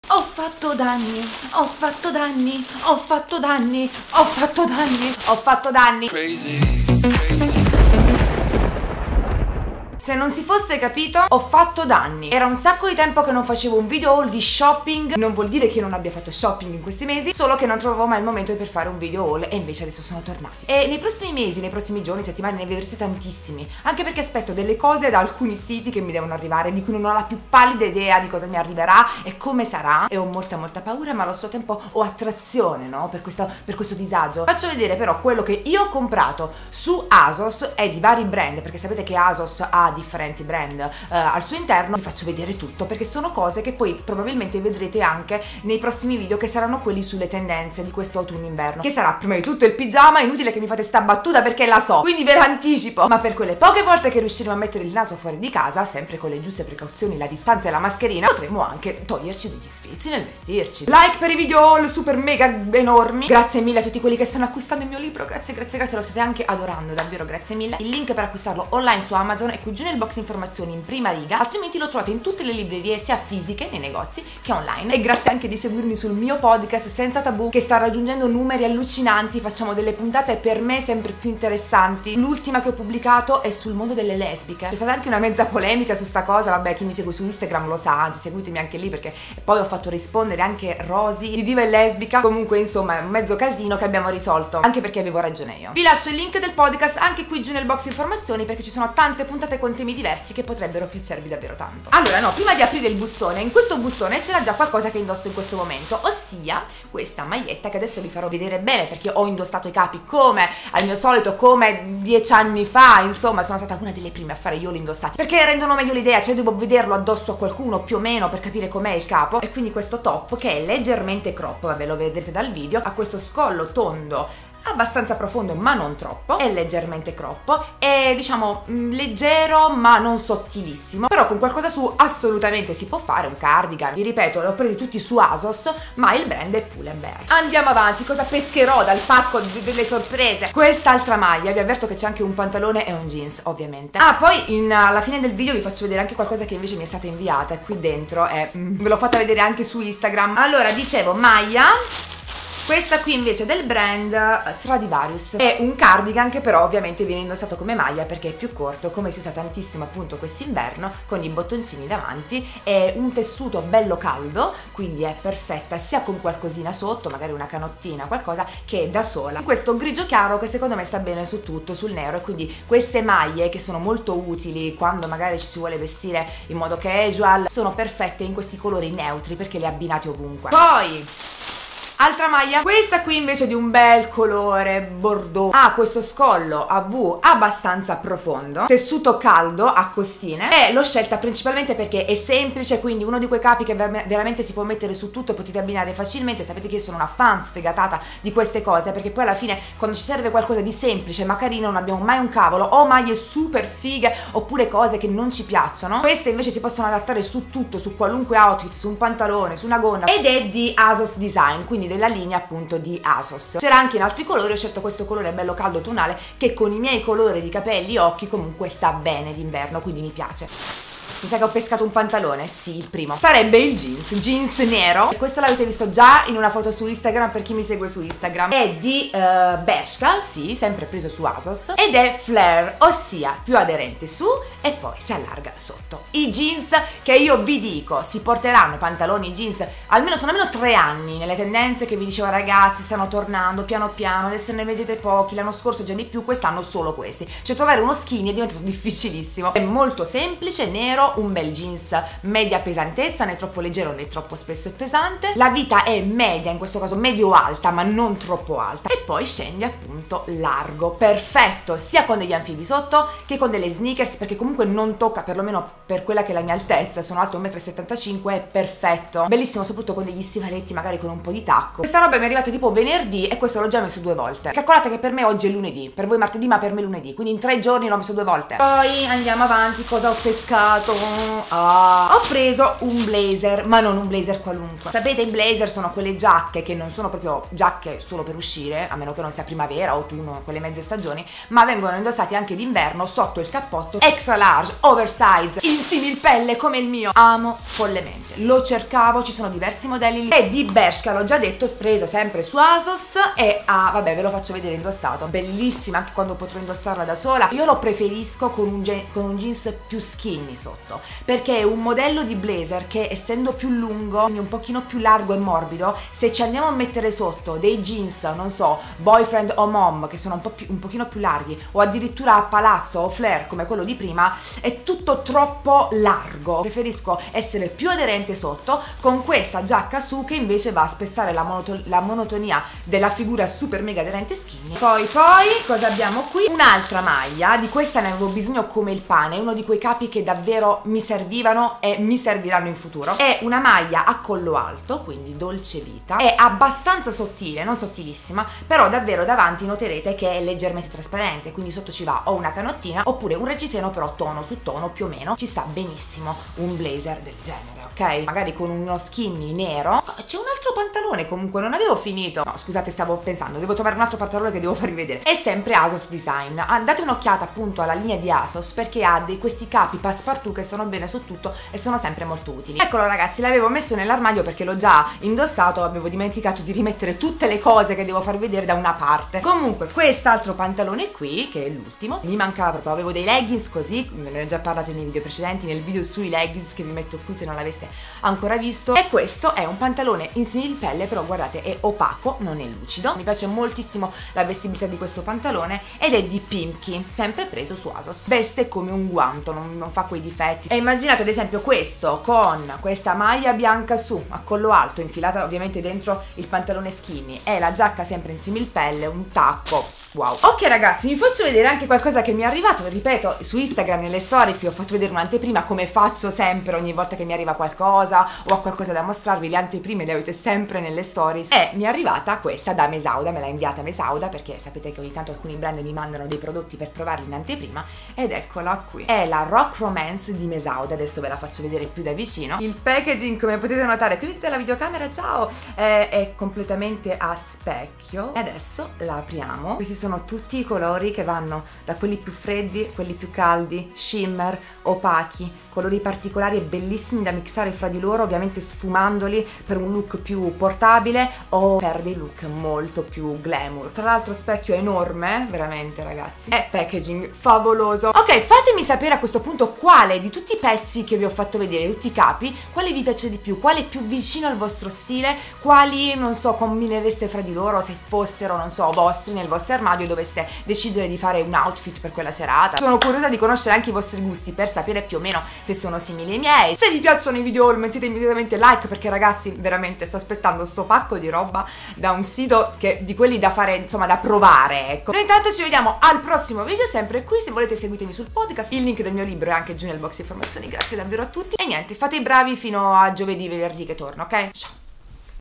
MEGA SHOPPING INDOSSATO 🛍 HO FATTO DANNI 🤦🏻‍♀️ 💸
🔥IL MIO LIBRO

Ecco tutto lo shopping che ho fatto in questi giorni su Asos. Pantaloni, blazer e maglie 🛍 Ho indossato tutto in questo try on haul. E, in più, la mia nuova palette di ombretti di Mesauda Rock Romance 💄